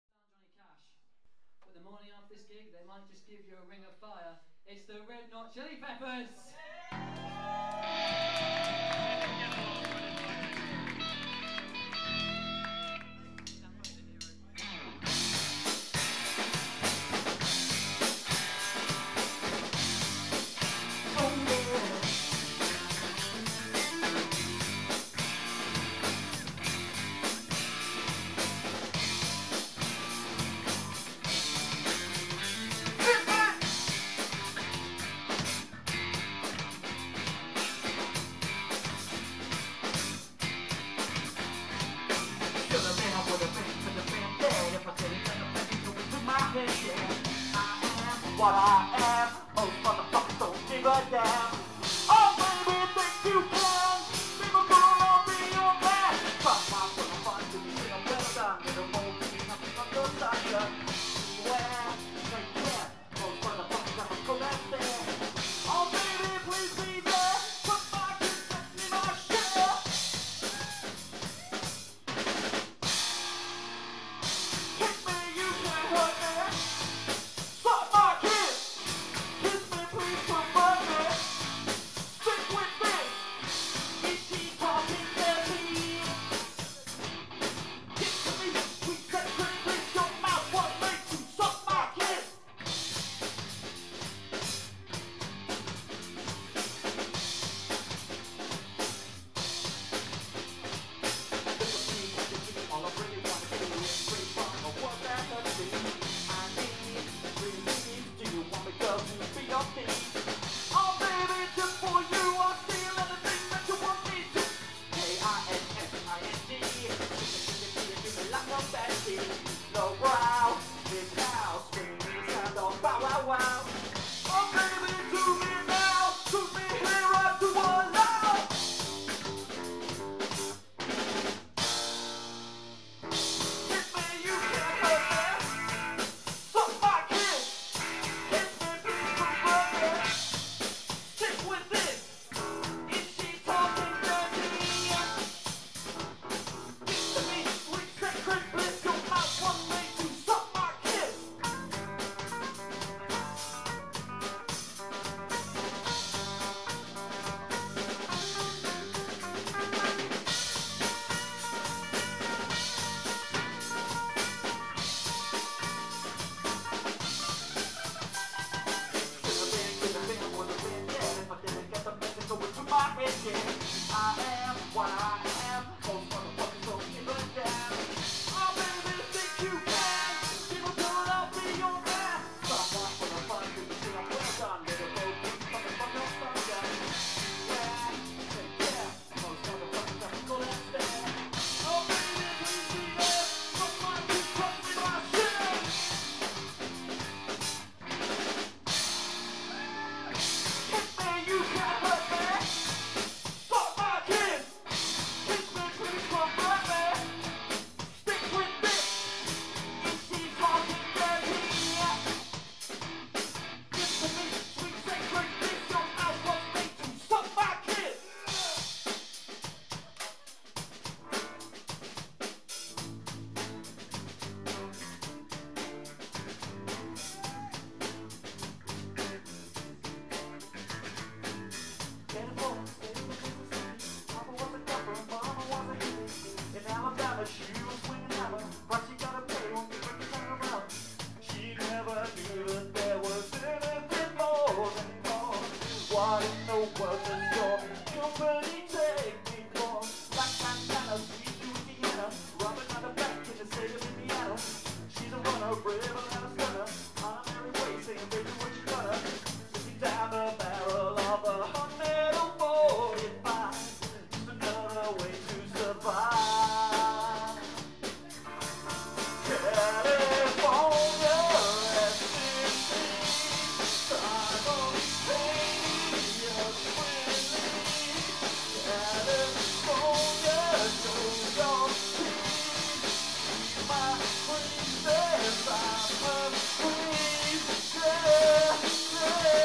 Red Not Chili Peppers Performance Part 1
Part of a Lead Performer unit at Access to Music Norwich. Played at the Brickmakers (with a terrible camera) Far from the perfect performance but it was a hell of alot of fun :)